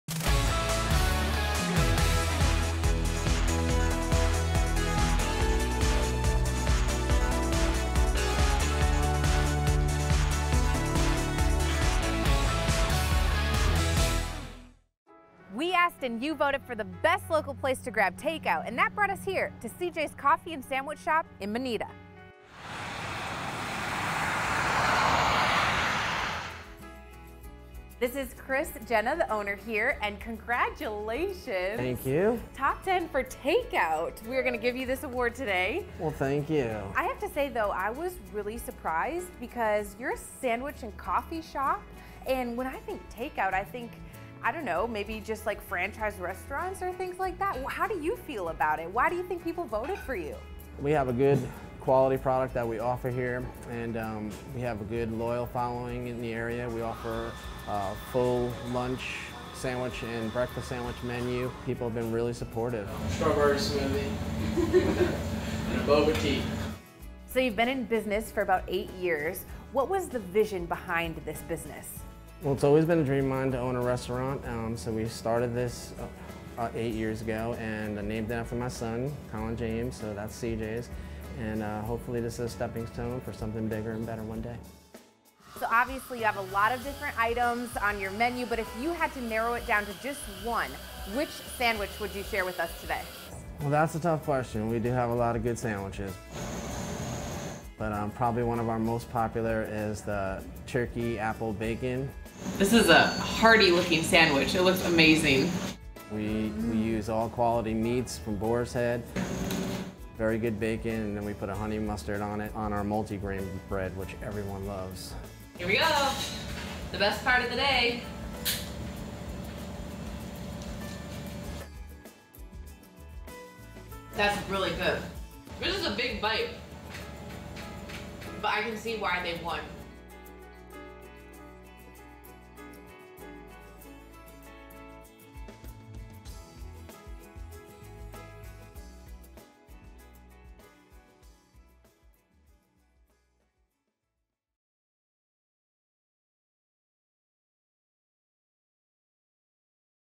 Winner announced for Top 10 Best Takeout
We asked and you voted for the best local place for takeout. Watch the video to see who won!